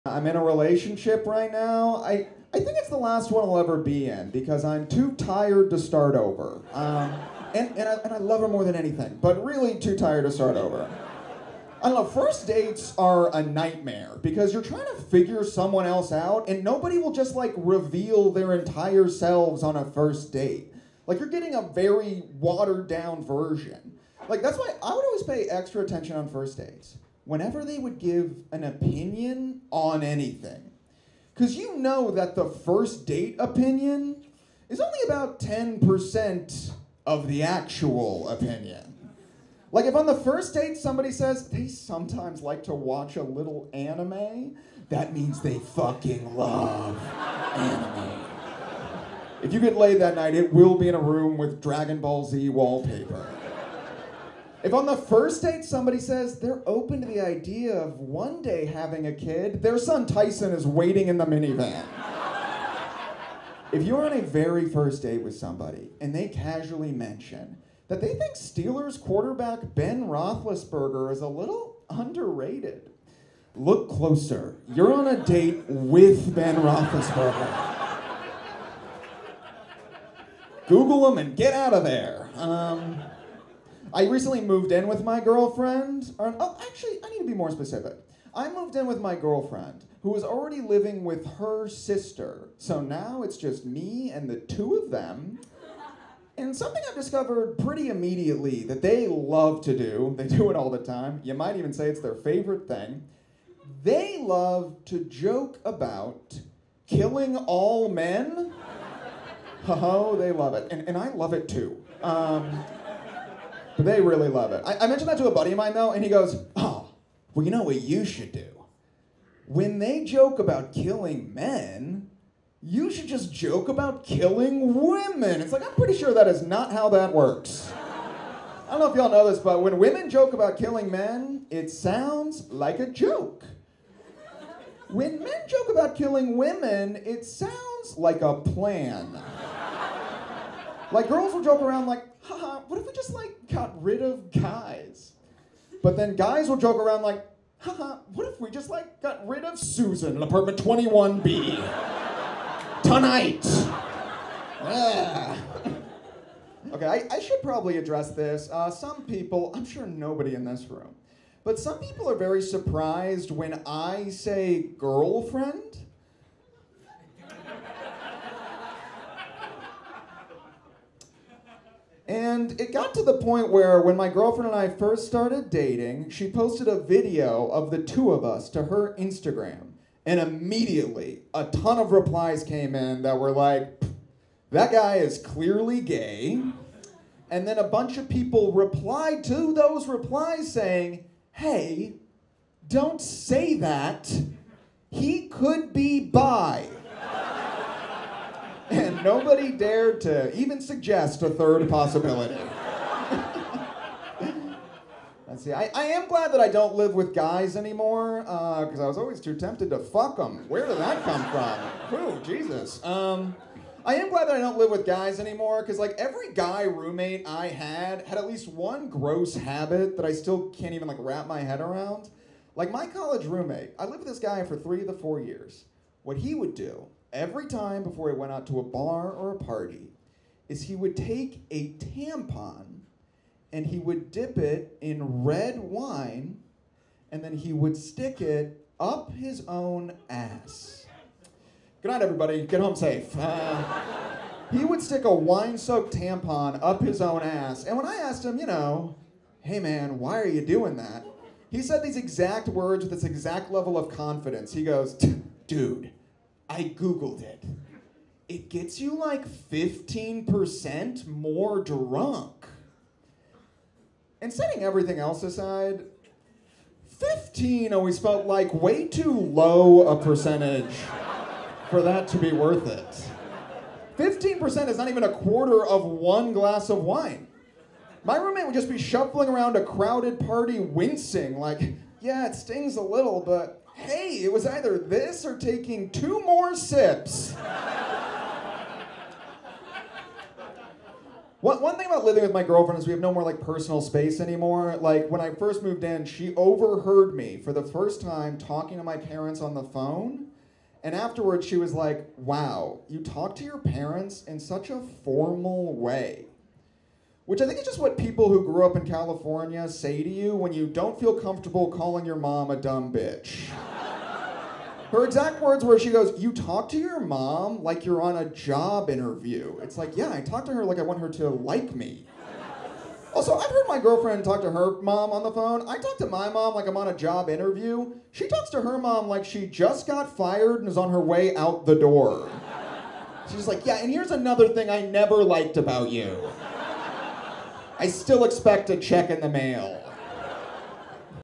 Dating is Exhausting | Robert Schultz | Standup Comedy
Comedian Robert Schultz talk about moving in with his girlfriend and why he’s happy to no longer have guys as roommates